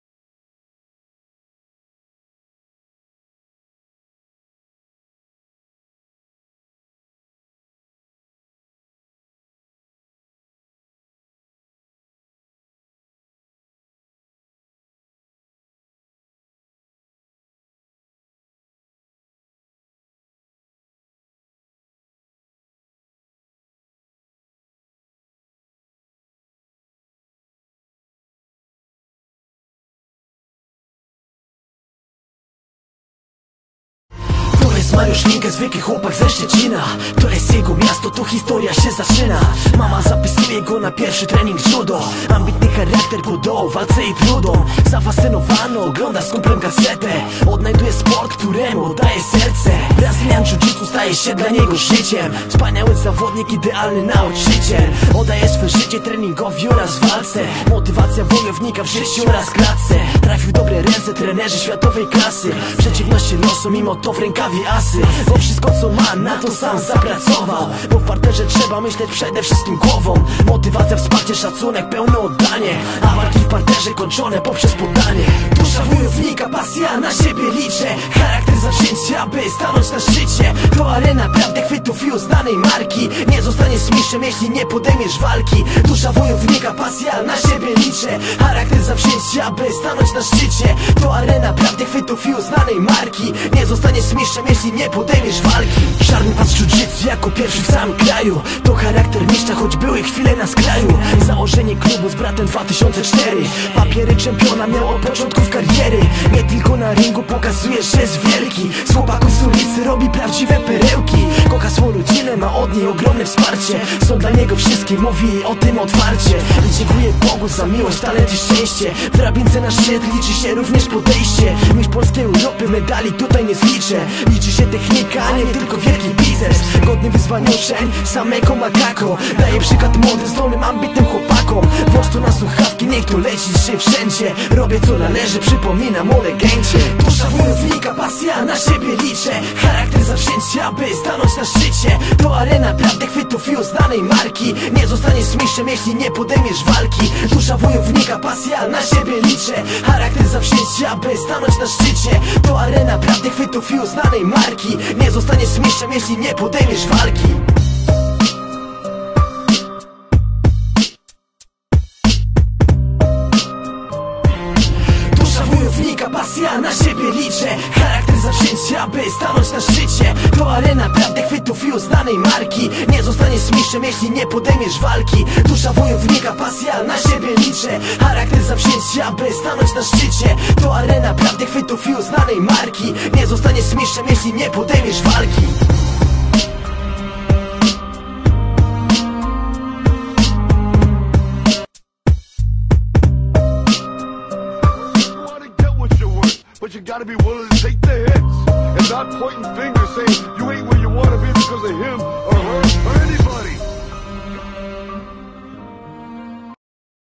Mariusz "MANOLO" Linke seminar at IGT
Master Mariusz Linke seminar at Immortals Gold Team Aberdeen
251-253 Great Northern Road
AB242AB